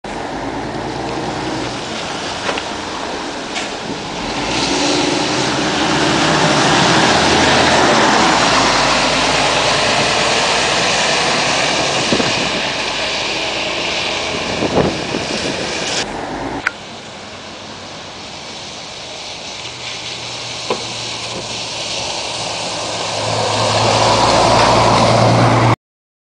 Bus-friendly speed hump
Buses crossing the speed hump. Traffic calming is particularly necessary nearby the the bus stop due to increased pedestrian movement and risk. [Stukenbrocker Weg, Oerlinghausen, Germany].

Autobusy przejeżdżające przez szykanę zawężającą z progiem spowalniającym. Fizyczne uspokojenie ruchu przy przystanku? Tak, bo tam piesi są najbardziej narażeni na potrącenie. [Stukenbrocker Weg, Oerlinghausen].